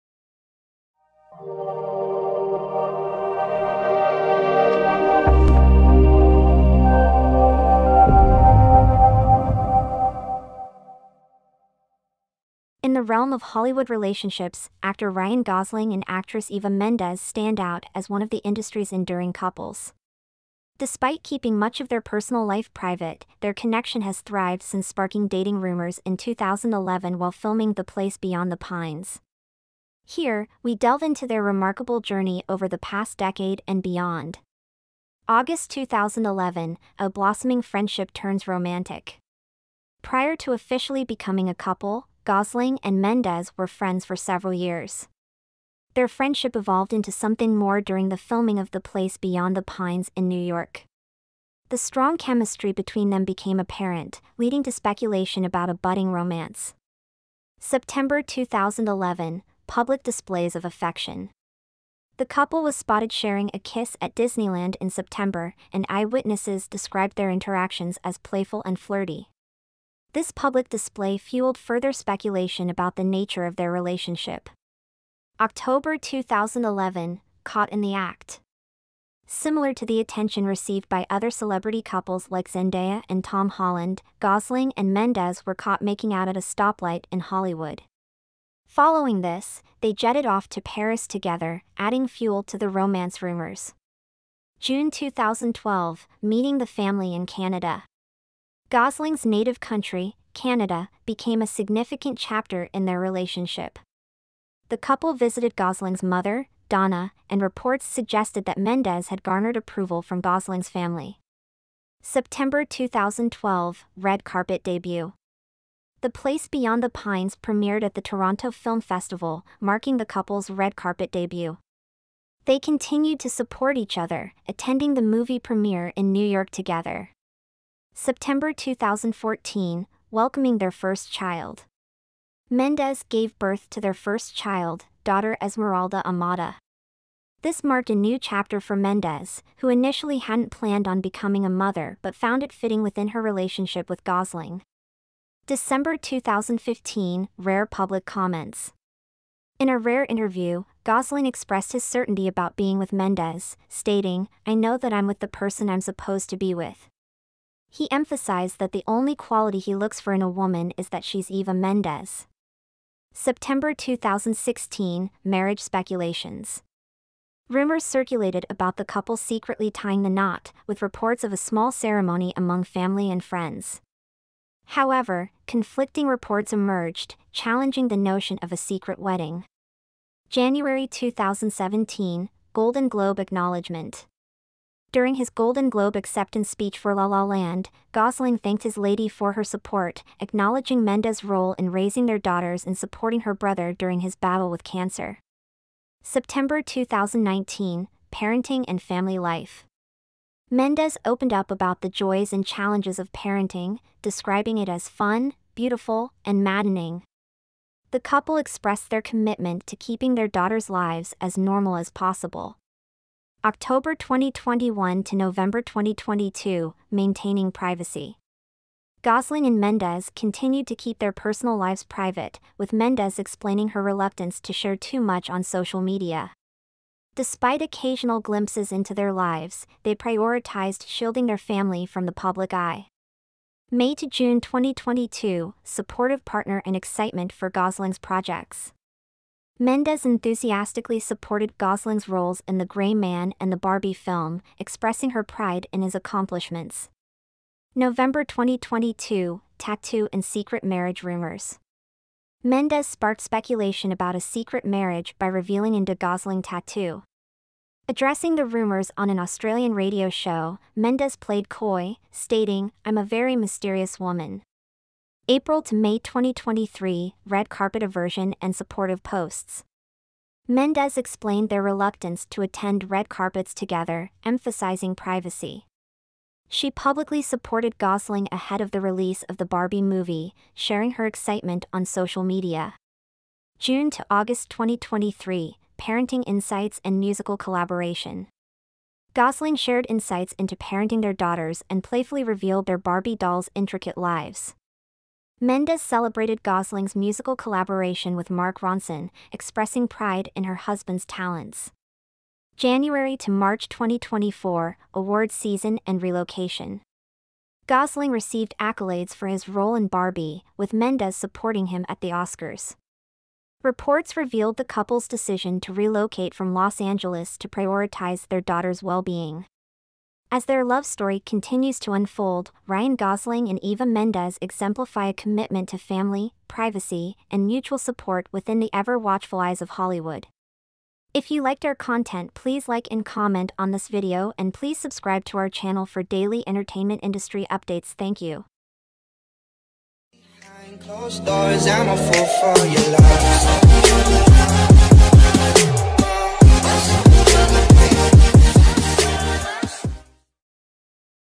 A Timeline of Ryan Gosling and Eva Mendes’ Super Private, Super Romantic Relationship
This video explores the remarkable journey of Ryan Gosling and Eva Mendes, one of Hollywood's most private yet enduring couples.

Starting from their on-set friendship in "The Place Beyond the Pines," the video dives into their blossoming romance